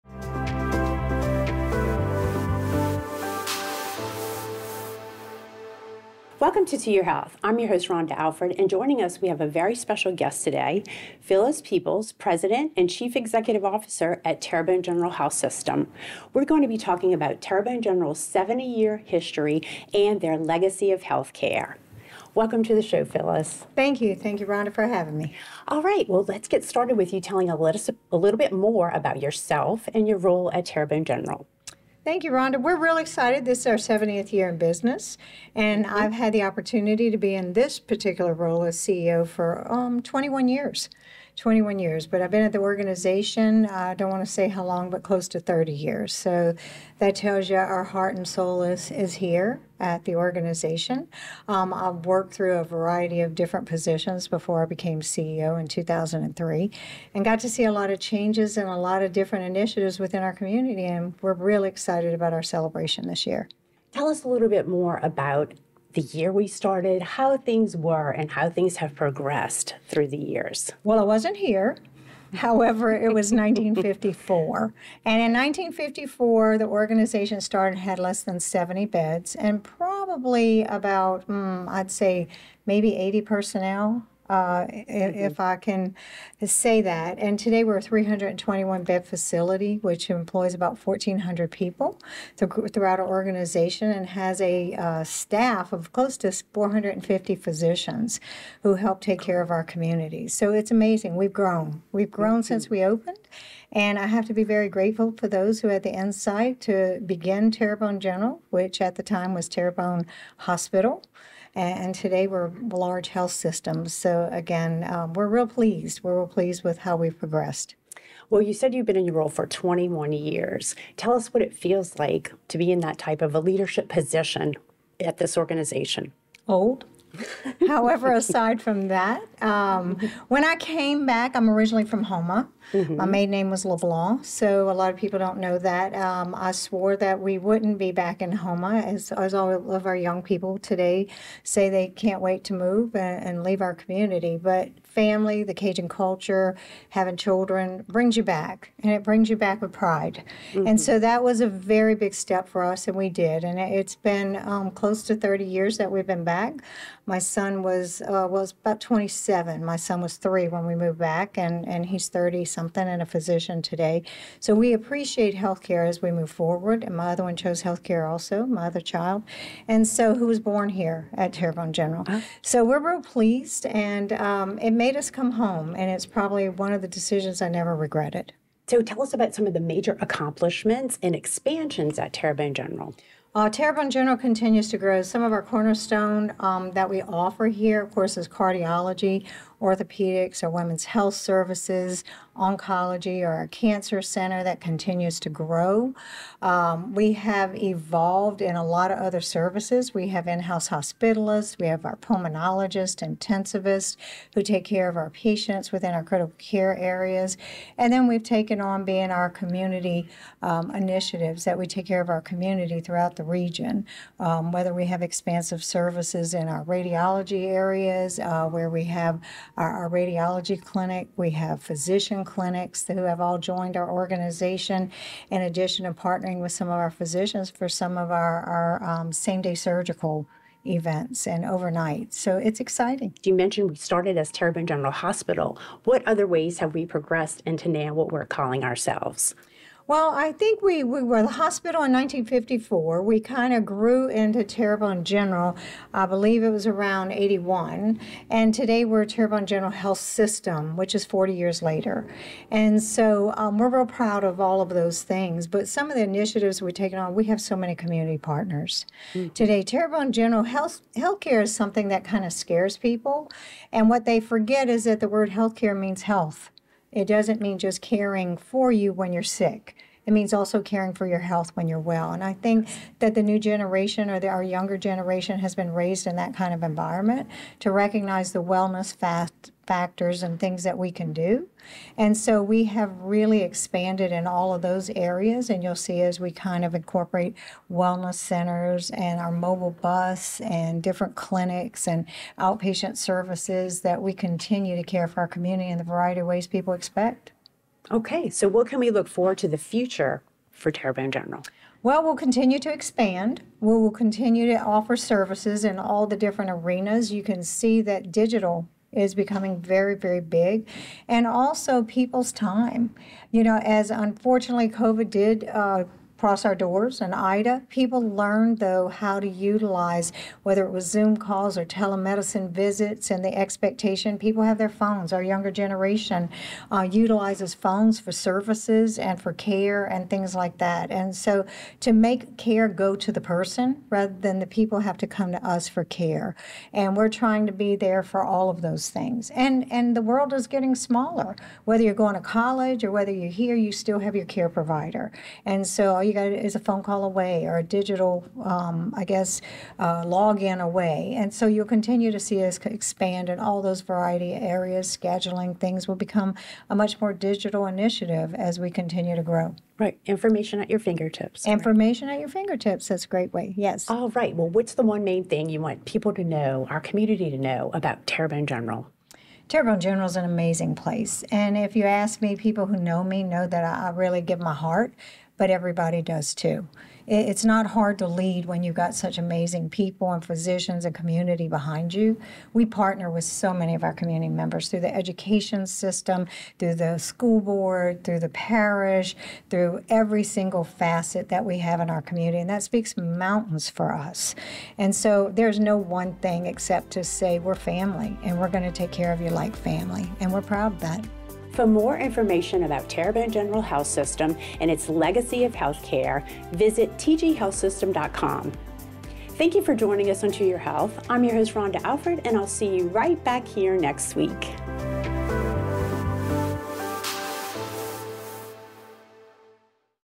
To Your Health - 70th Anniversary
Joining us on this very special episode is Phyllis Peoples, President and CEO of Terrebonne General Health System, who will share with us more about Terrebonne General’s rich 70-year history and legacy of healthcare.